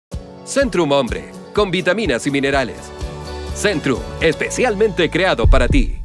Centrum Hombre: Hay uno especialmente creado para ti
Centrum Hombre, con vitaminas y minerales

Centrum, especialmente creado para ti

Centrum, el multivitamínico N°1 en ventas en el mundo

*IQVIA Global OTC Insight - Ventas en Valor entre Julio 2018 y Junio 2019.

Si quieres notificar un acontecimiento adverso o un efecto secundario de alguno de nuestros productos, llama al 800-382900 para que podamos atenderte de la forma más personalizada.